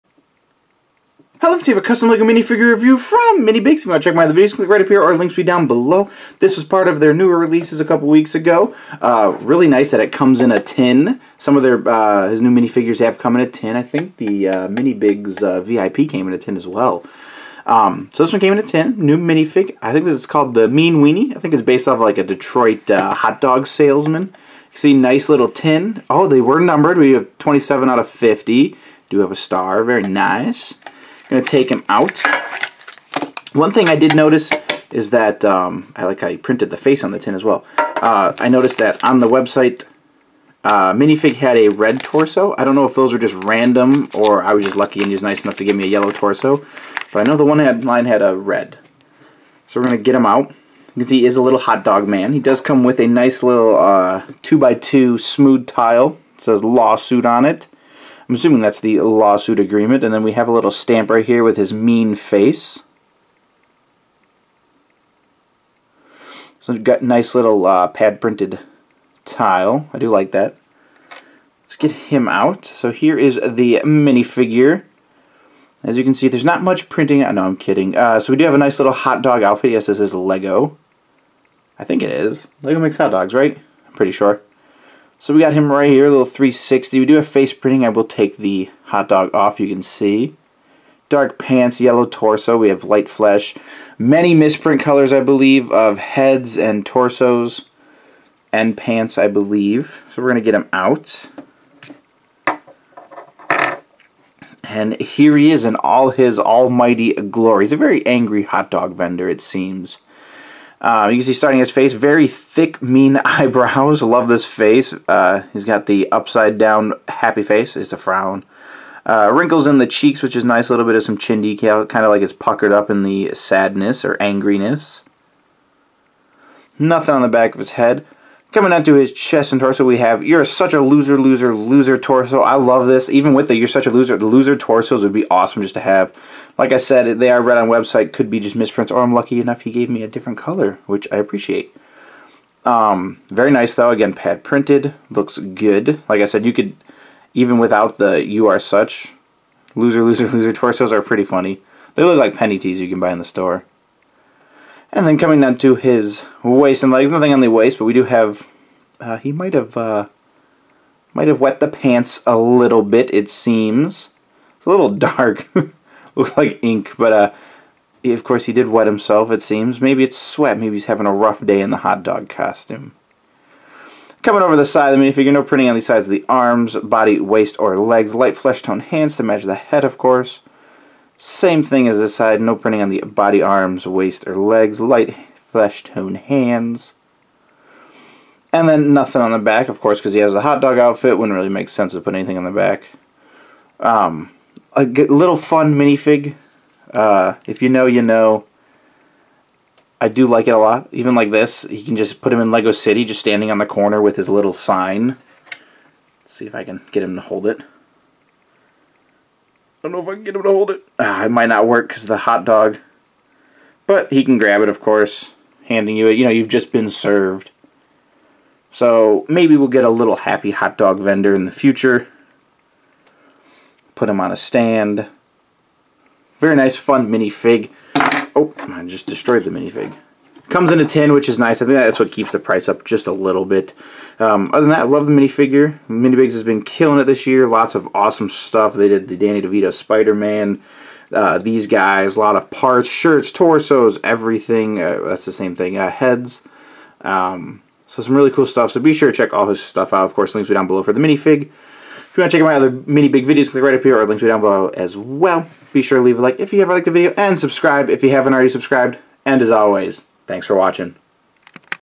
Custom Lego Minifigure Review: miniBIGS Mean Weanie
GET YA HOTDOGS! Cold, hot, new, or old. FRESH HOTDOGS HERE!  If you know, then you know.  This is a fun minifigure coming out of miniBIGS, even has the lawsuit ready for you to get served some delicious hotdogs!  Even misprint colors for the fruit dog man!